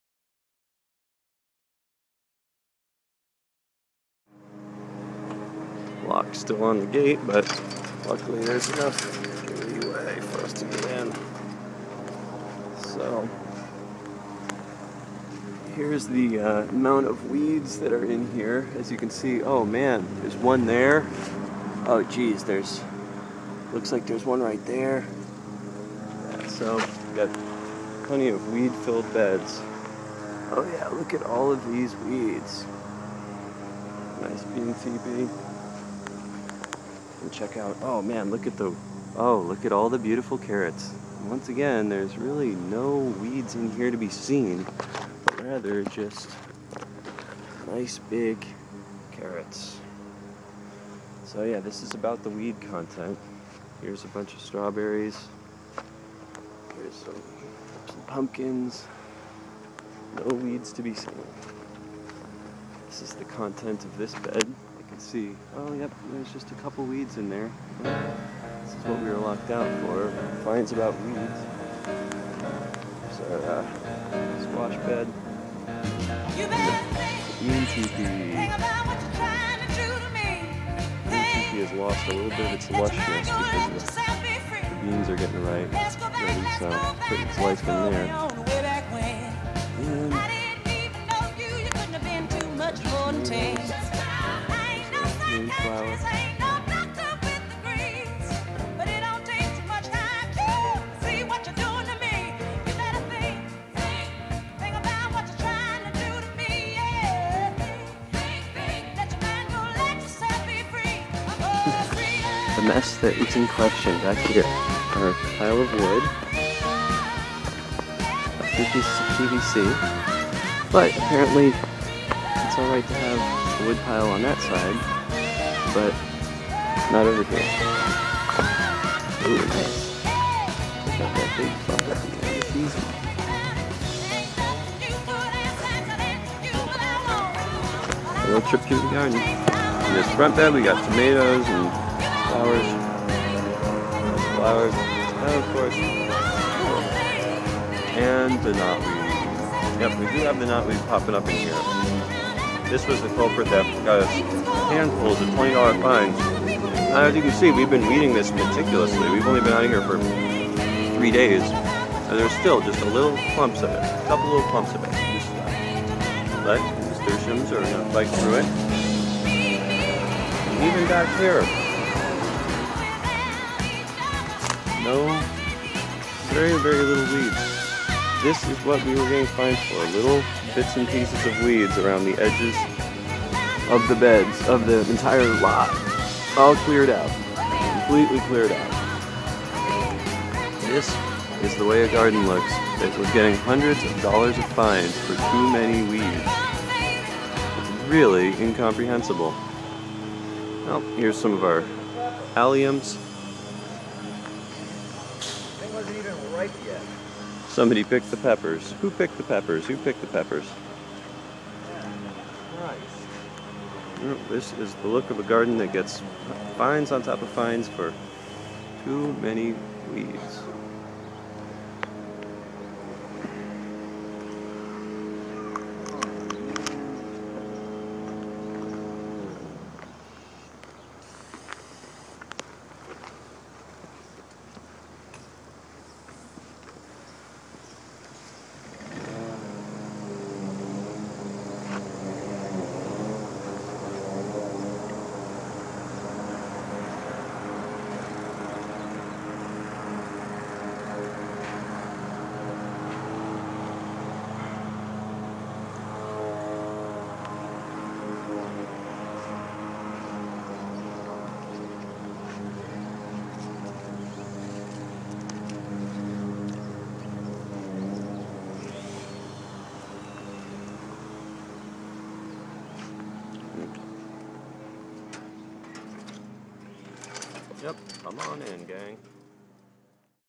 "Excessive Weeds"
A trip through Fertile Underground "behind the gate" after getting locked out of our rented garden for hundreds of dollars worth of illegal "fees" levied for "weeds" and materials "not for garden use".  After the first half dozen $20 fees, we decided to stop paying, because they were baseless.  We discovered that we had really no chance to keep the garden in a way that did not bring "fees" from this landlord, stepping over the bounds, with his unannounced inspections, grainy digital images of five inch high weeds, and bills ranging up to $600, in a garden which regular visitors marvel at.
     Our group of 30 members do an extraordinary job caring for this land, and have built this garden from scratch.  The levels of disrespect we have received from the landlord, Yarrow Thorne of Thornedale LLC, are just baffling, and we have been resolved to laugh them off for what they are- the asinine inflictions of a controlling land-owner, who would be better off to improve his own garden, than try to skim 20s of dollars off a community group who have already paid him $2600 for use of the space in a peaceful manner.
  We request that Mr. Thorne attempt to be a good neighbor and let us live out the rest of the season in peace.